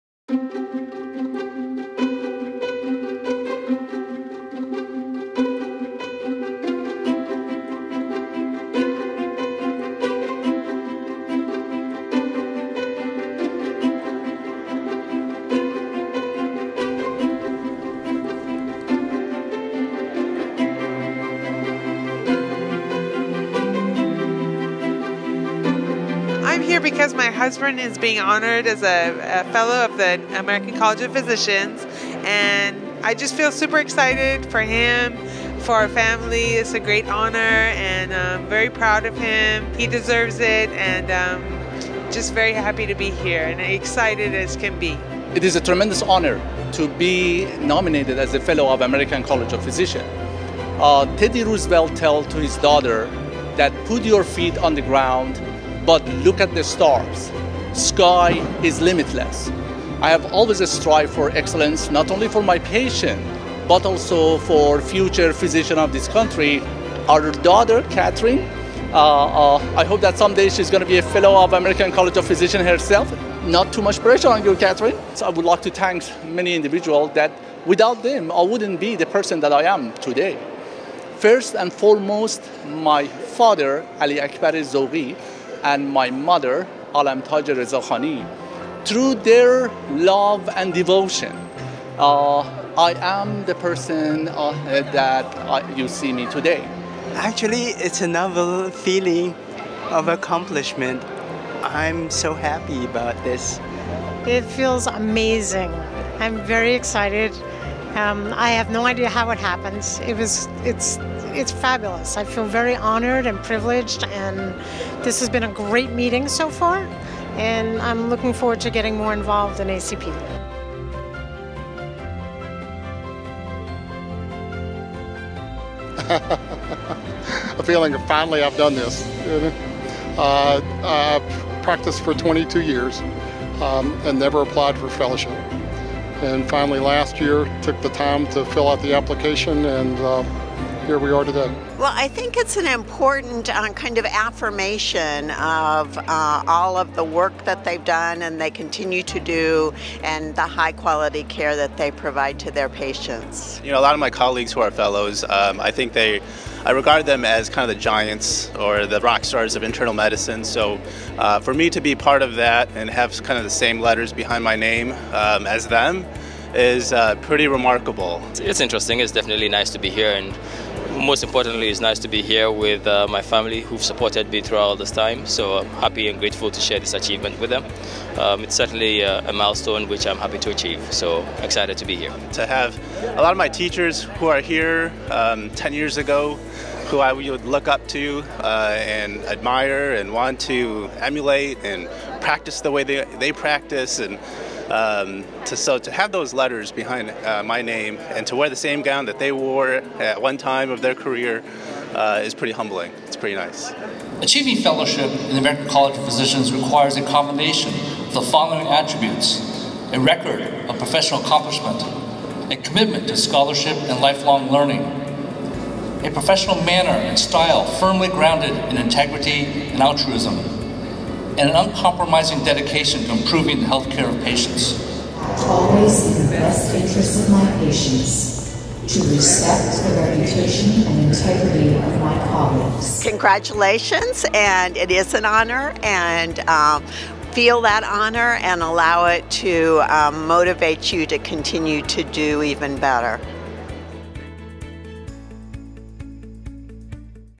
American College of Physicians Ceremony, San Diego 2017
ACP Convocation Ceremony held on March 30, 2017 at the San Diego Convention Center.